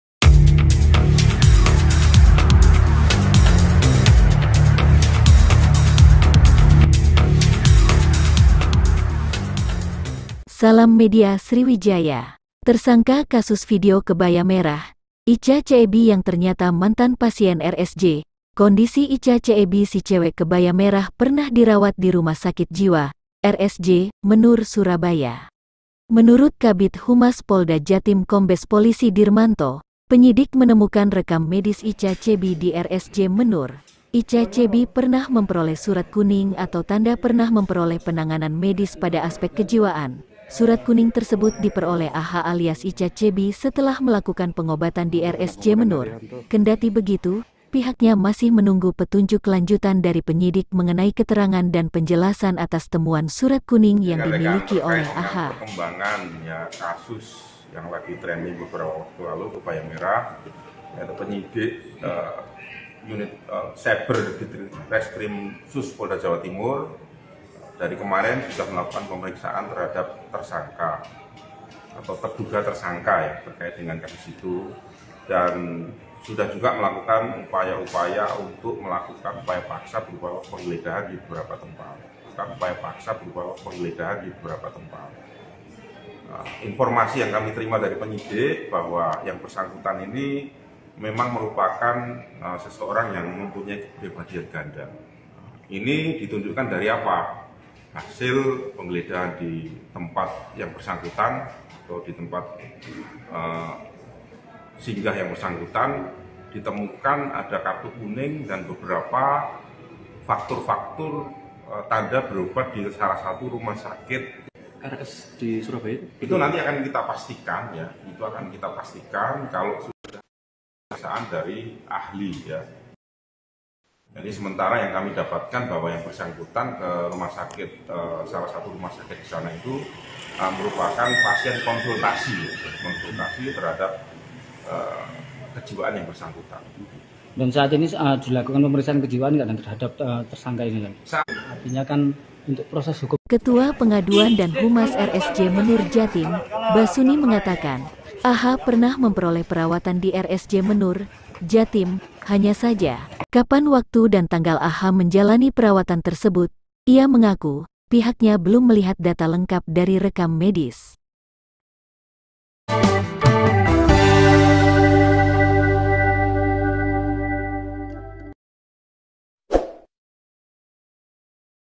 POLDA JATIM : PEMERAN WANITA VIDEO KEBAYA MERAH PERNAH DIRAWAT DI RSJ MENUR JAWA TIMUR
SURABAYA,MEDIASRIWIJAYA - Kondisi Icha Ceeby si cewek kebaya merah pernah dirawat di Rumah Sakit Jiwa (RSJ) Menur Surabaya

Menurut Kabid Humas Polda Jatim Kombes Pol Dirmanto, penyidik menemukan rekam medis Icha Ceeby di RSJ Menur.
Icha Ceeby pernah memperoleh surat kuning atau tanda pernah memperoleh penanganan medis pada aspek kejiwaan.

Surat kuning tersebut diperoleh AH alias Icha Ceeby setelah melakukan pengobatan di RSJ Menur.

Kendati begitu, pihaknya masih menunggu petunjuk lanjutan dari penyidik mengenai keterangan dan penjelasan atas temuan surat kuning yang dimiliki oleh AH.

SUMBER ; instagram